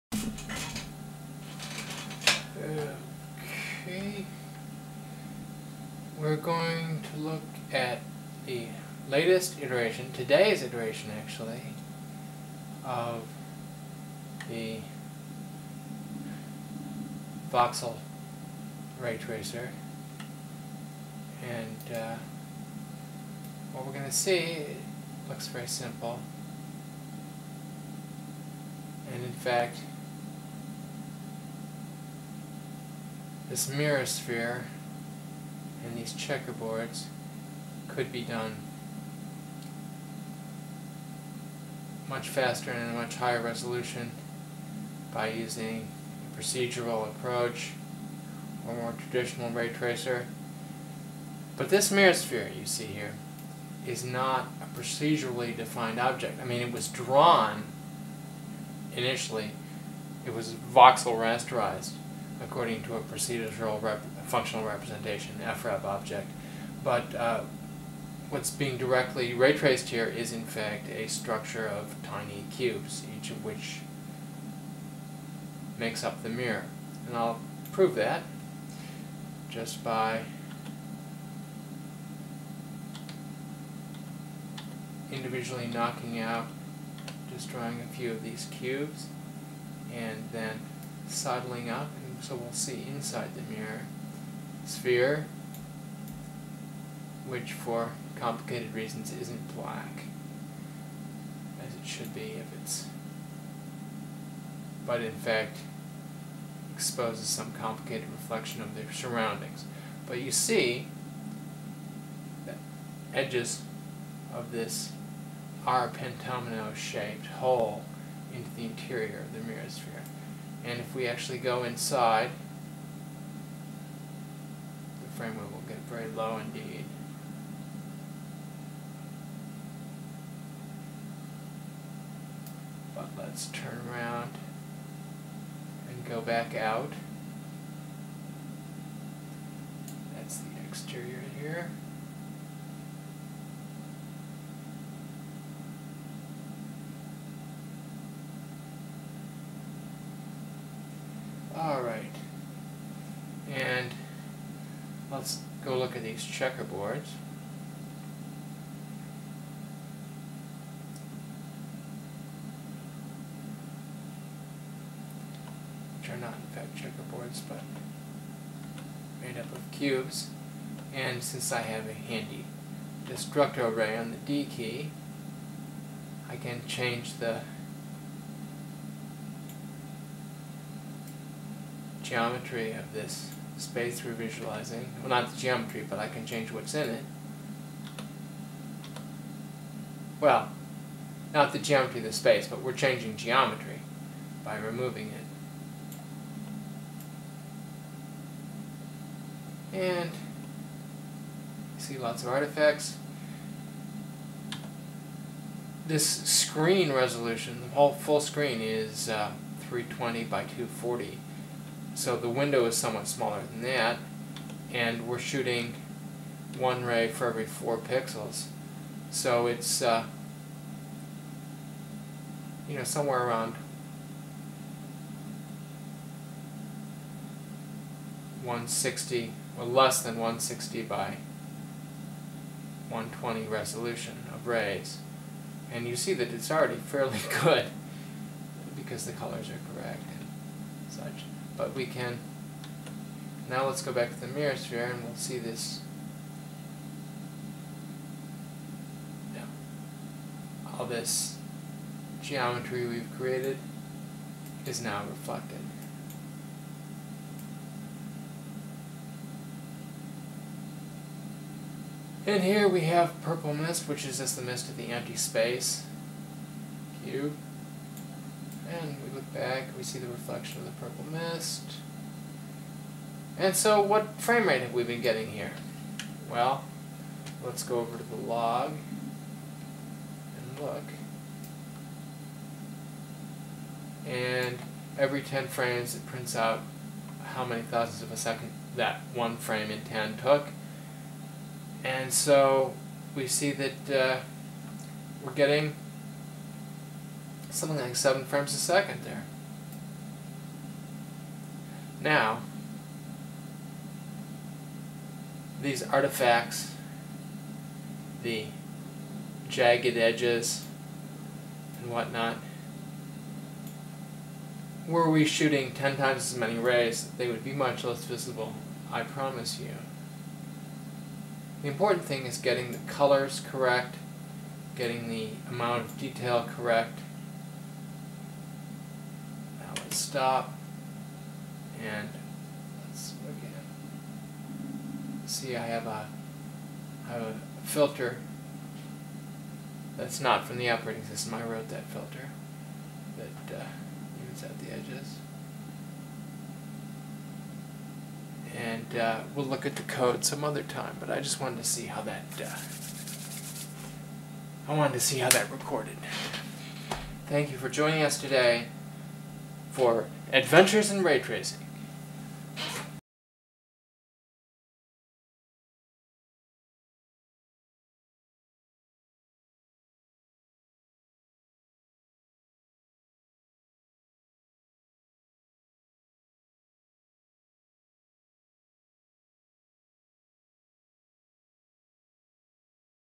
adventures in raytracing
filmed off the monitor in real time with canon gl1 minidv camera. deinterlaced and edited in cinelerra.

correction: "P" pentomino, not "R".